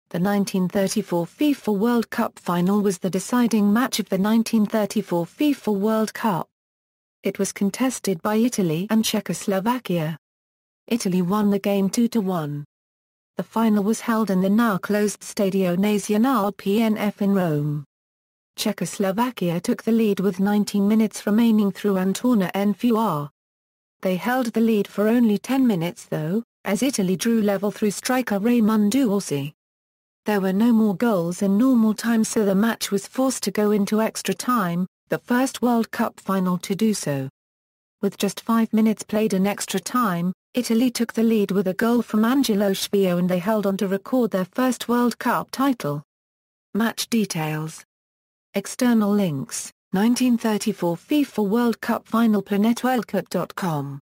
1934 FIFA World Cup Final
The 1934 FIFA World Cup Final was the deciding match of the 1934 FIFA World Cup. It was contested by Italy and Czechoslovakia. Italy won the game 2-1.
The final was held in the now-closed Stadio Nazionale PNF in Rome. Czechoslovakia took the lead with 19 minutes remaining through Antonín Puč. They held the lead for only 10 minutes though, as Italy drew level through striker Raimundo Orsi. There were no more goals in normal time so the match was forced to go into extra time, the first World Cup final to do so. With just five minutes played in extra time, Italy took the lead with a goal from Angelo Schiavio and they held on to record their first World Cup title.